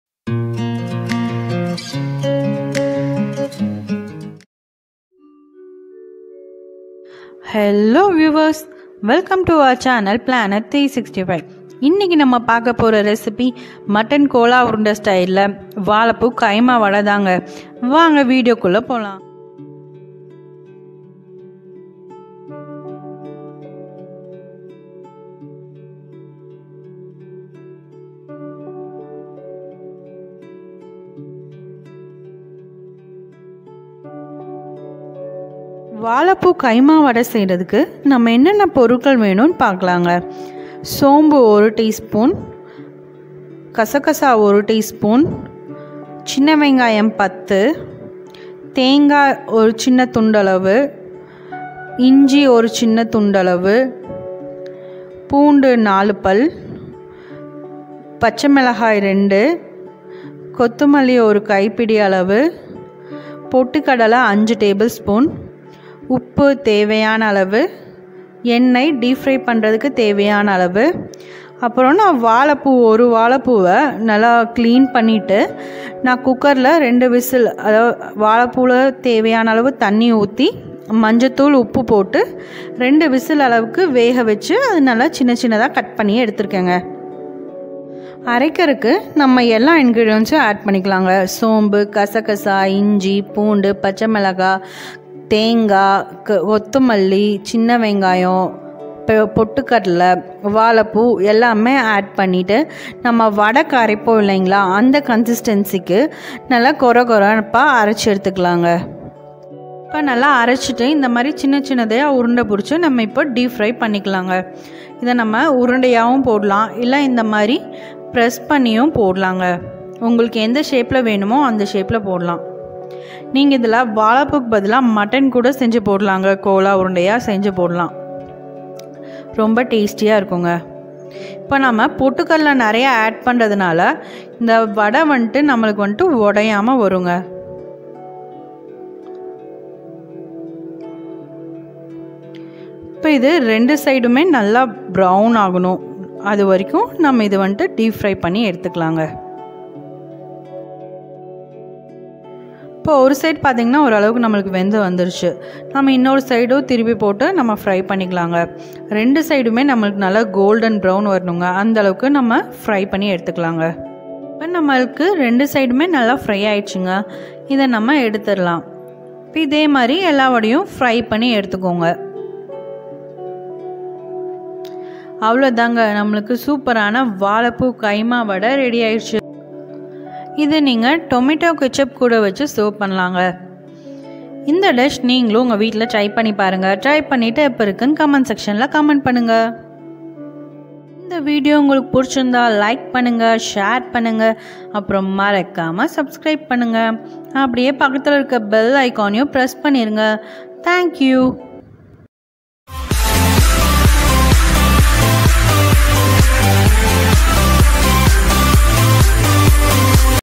Vazhaipoo Kaima Vadai | Tamil | Cooking
This Video is about making Vazhaipoo Kaima Vadai.

Ingredients:

1. Banana flower
2. Small Onion - 10
3. Garlic - 4
4. Ginger
5. Green Chilli - 2
6. Coconut - 2 tbs
7. Roasted Gram - 5 tbs
8. Anise - 1 tsp
9. Poppy Seeds - 1 tsp
10. Coriander leaves
 11. Salt
12. Oil